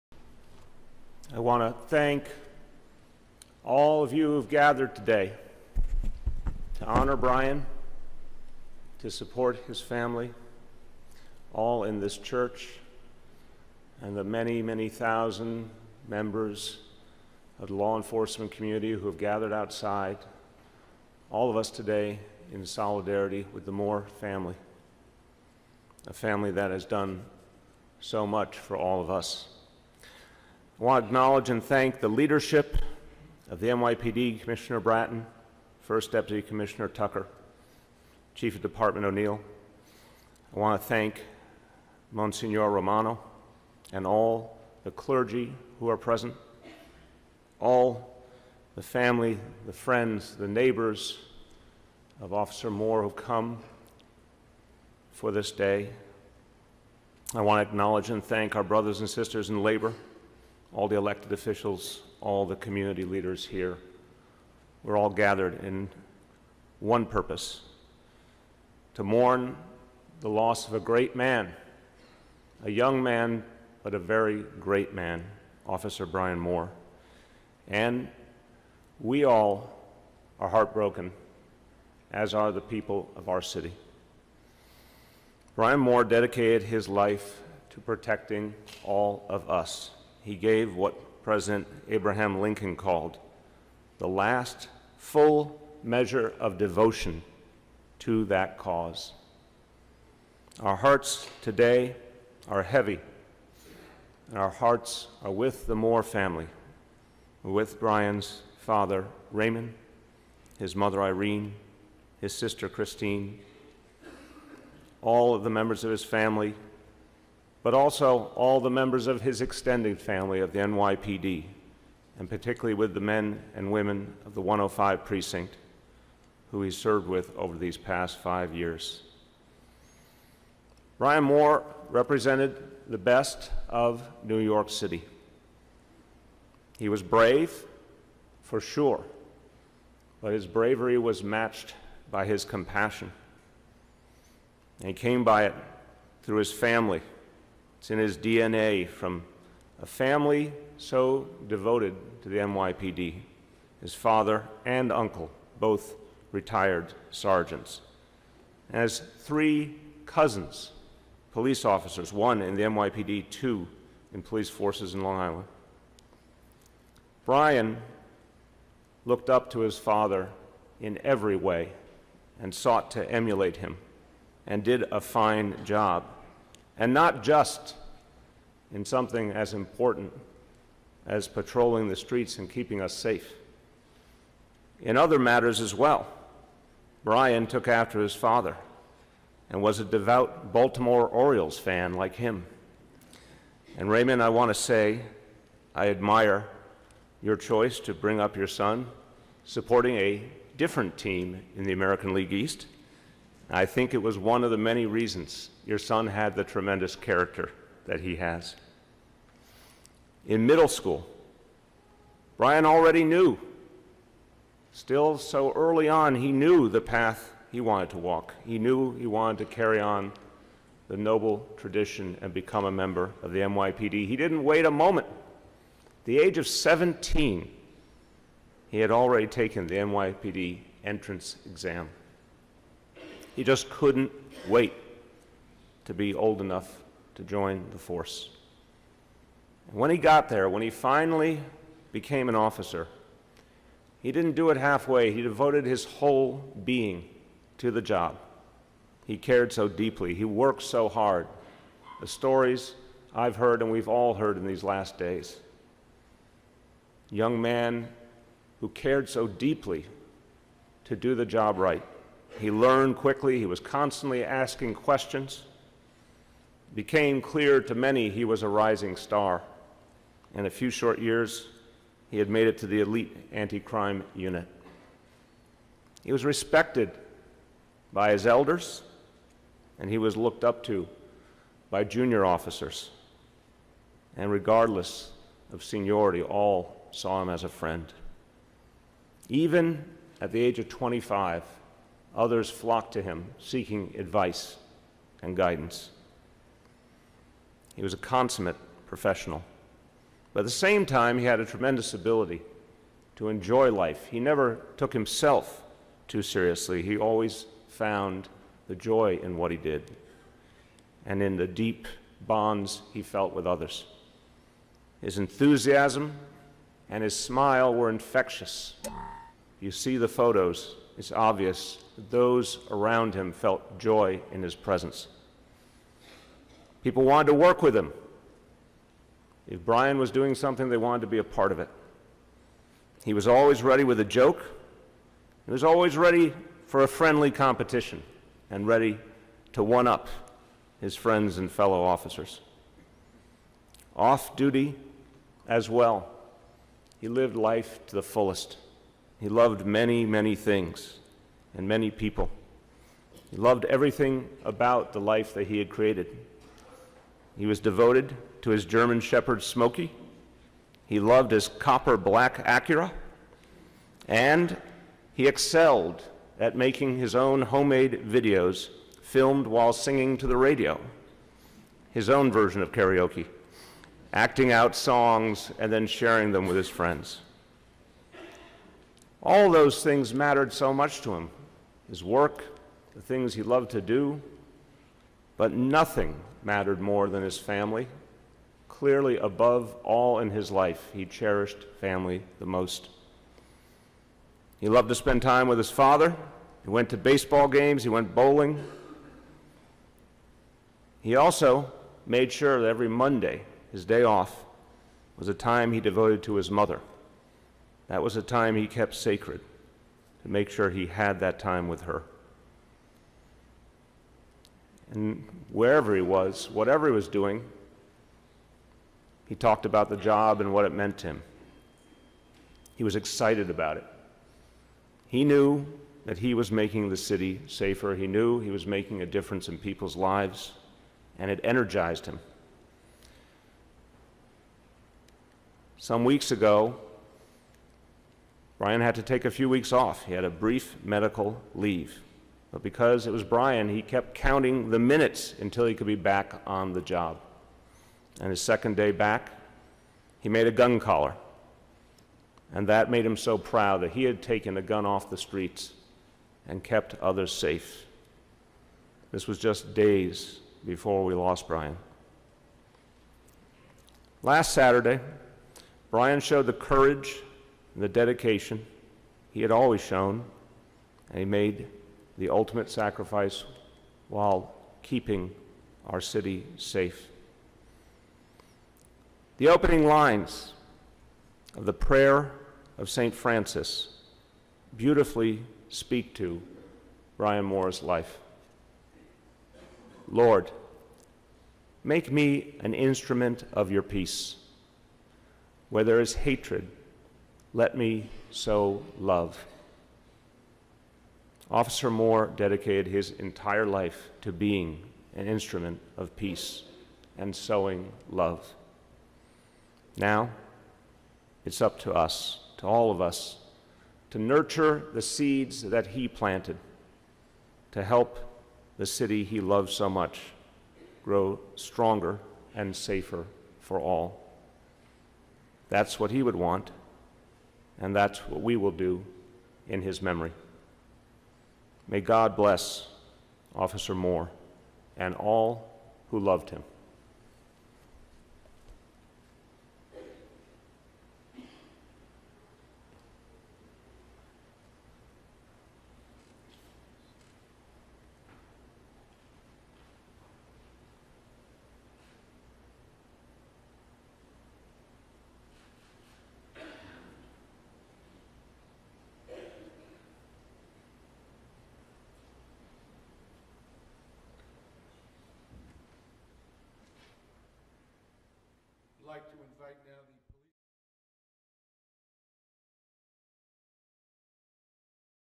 Mayor de Blasio Remarks at Officer Brian Moore's Funeral (FULL VIDEO)
Mayor Bill de Blasio remarks at Officer Brian Moore's funeral. Moore, an officer in the NYPD, was killed last week in a line of duty shooting.